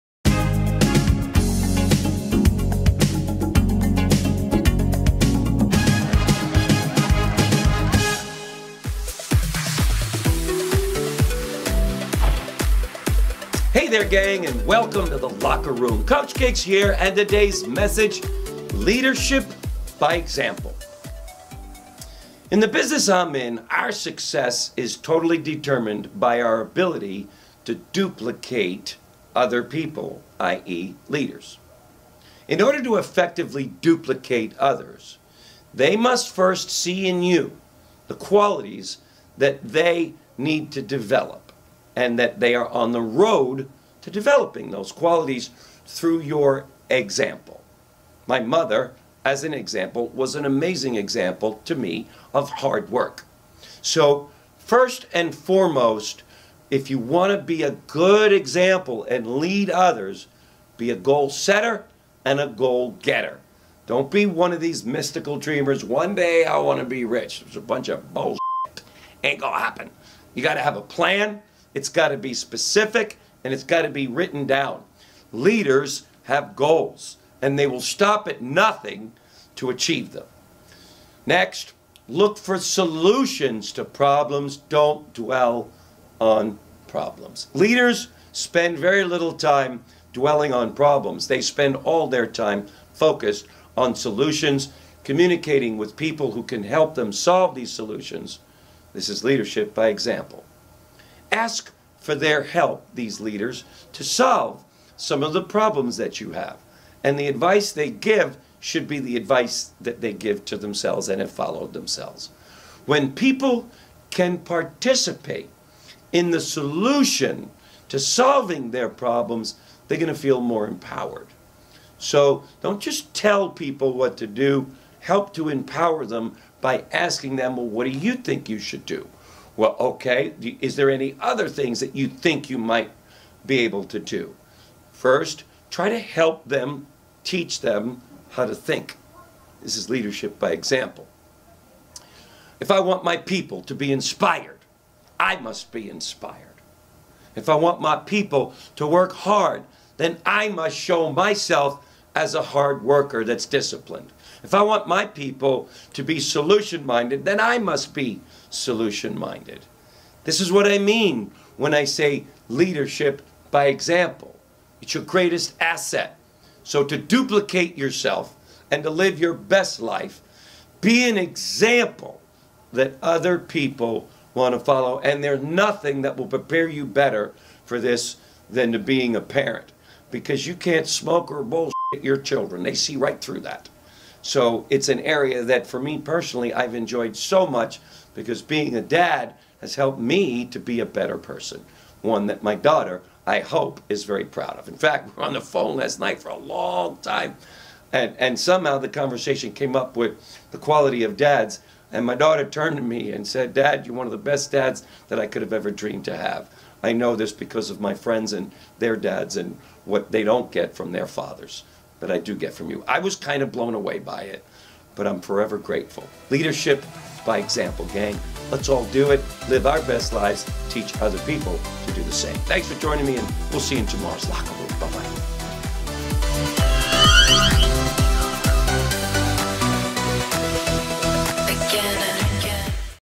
Leadership by Example - Coach Gig's Daily Locker Room
In the business I'm in, our success is determined by our ability to duplicate leaders.
In order to effectively duplicate others, they must see in you those qualities that they are on the road to developing.
First…